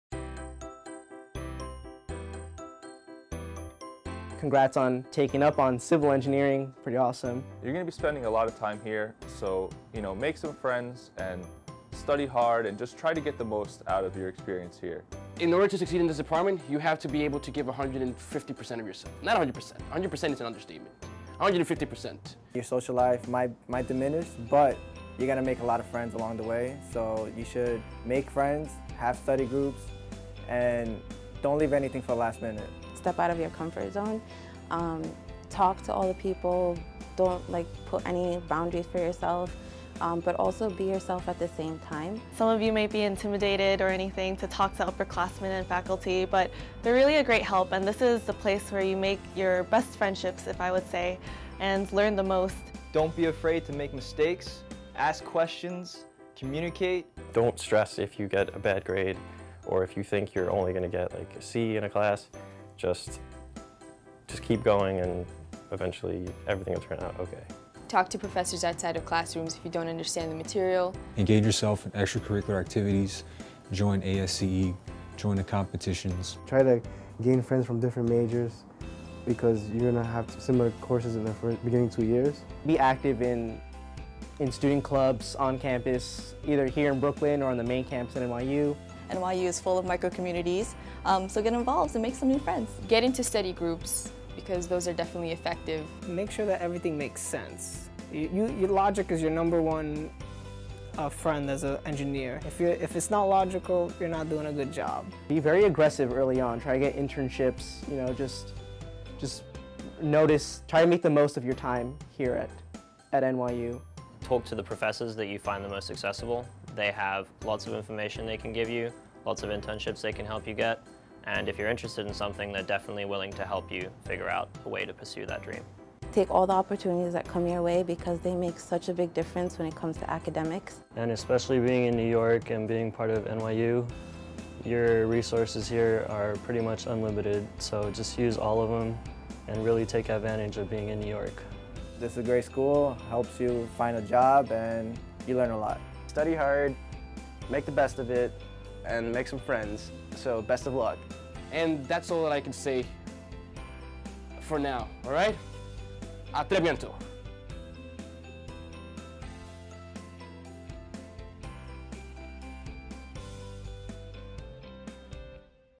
Advice for First-Year Students
Graduating students from NYU Tandon's Civil and Urban Engineering Department give advice to first-year students.